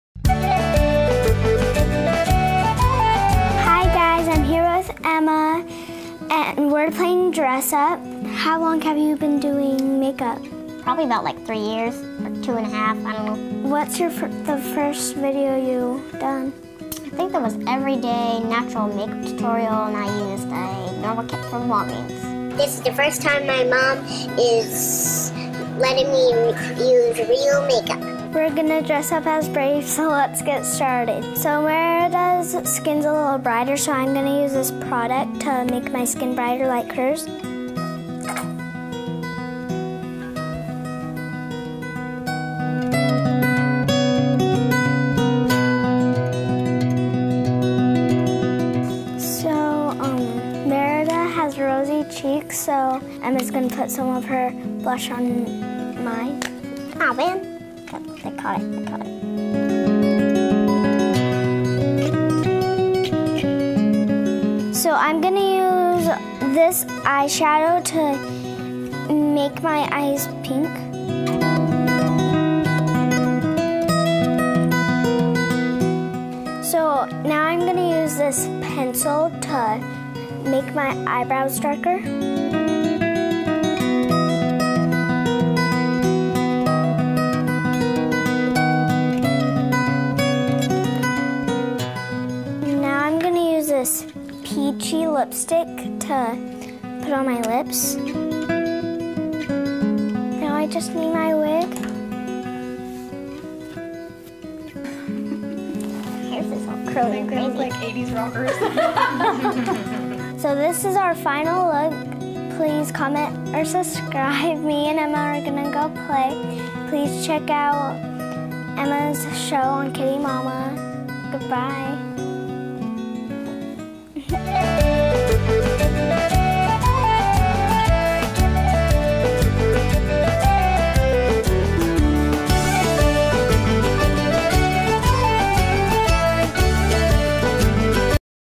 Brailee's First Make-up Tutorial with Emma!!
Hi everyone!! We are so excited for you guys to see today's video! We love watching Emma, from KittiesMama, do her Monster High make-up tutorials and we were so happy to be able to have her on our show! The girls had a blast playing dress up! I don't think this will be Brailee's last make-up tutorial!:) Thank you so much Emma and family! We loved meeting you! We love you all!!